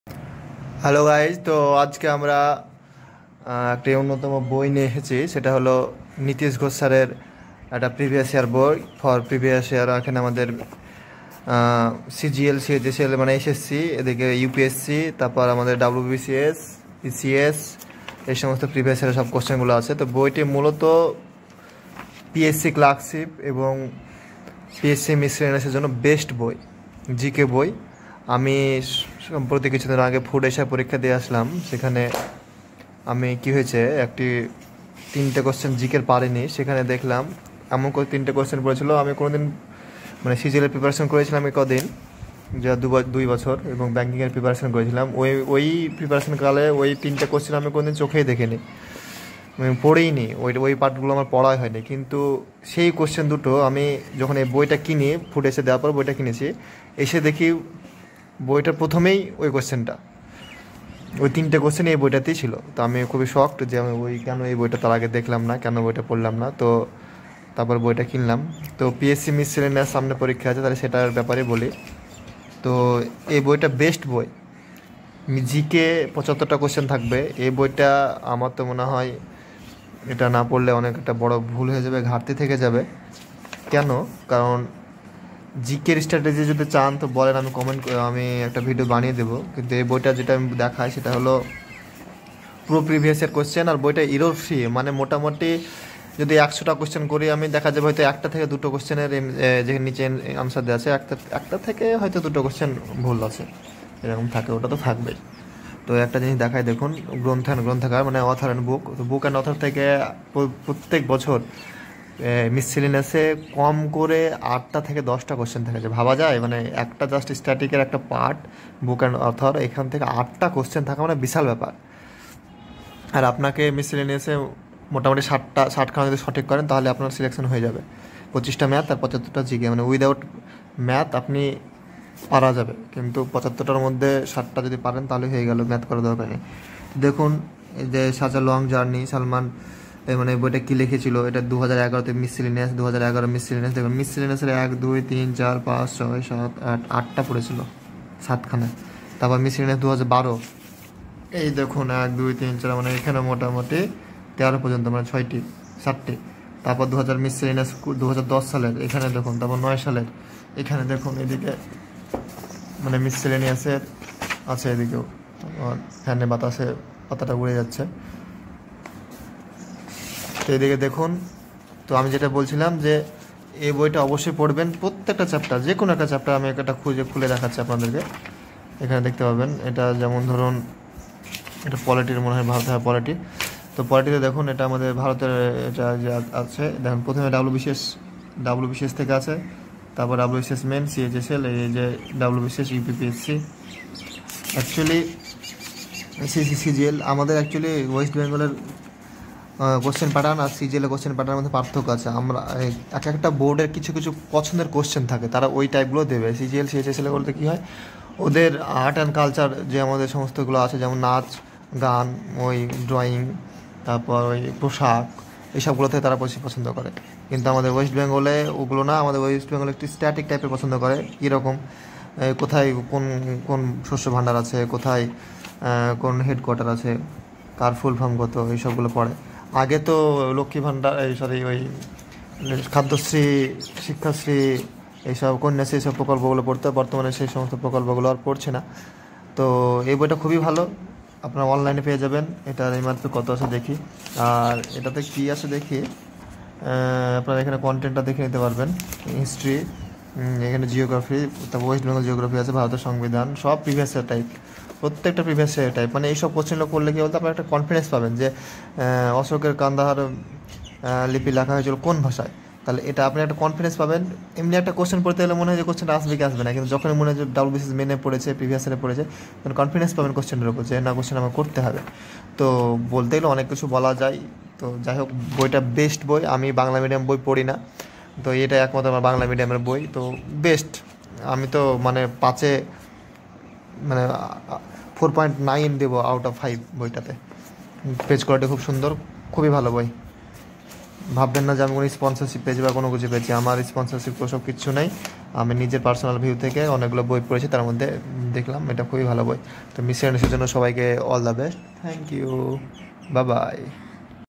Objective General Knowledge by Nithish Ghosh Book Review...
in this video I want to review a book objective general knowledge by Dr Nitish Ghosh this book is best for miscellaneous flagship serial